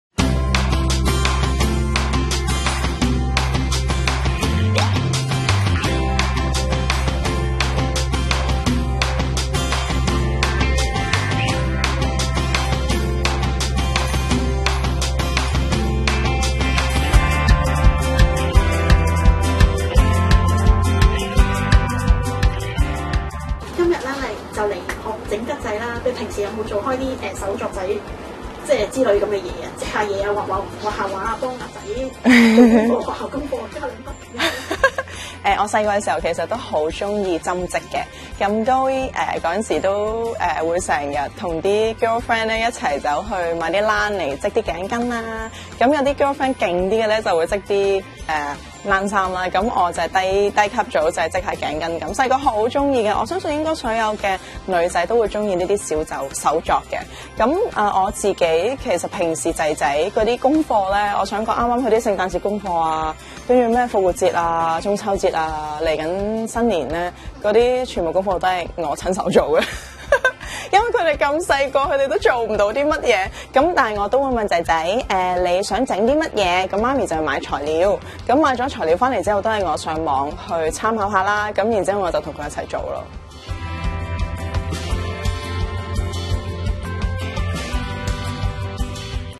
桔星高照 岑潔儀
織織復織織，潮流興鈎織。忙於打理自家飾物品牌Inizo的岑潔儀（Zoey），趁新年走到銅鑼灣學織桔，取其「吉星高照」好意頭。

「好好玩，鈎織好環保，織完又可以裝飾家居。我學識後教兩個仔，可以讓他們坐定定，未嘗不是一件好事。」

新年期間適逢情人節，Zoey投訴老公Dixon好現實，嫌鮮花太快凋謝而從不送花，「可能叫他改送親手織的花吧。」
無論鮮花或織花，最緊要有花收吖嘛！

喜歡做手作仔的岑潔儀，讀書時期經常買毛冷織頸巾，她笑言自己是低班，比較勁的朋友仔會織冷衫。而生了兩個小朋友後，近年她經常出馬，幫兒子做勞作功課。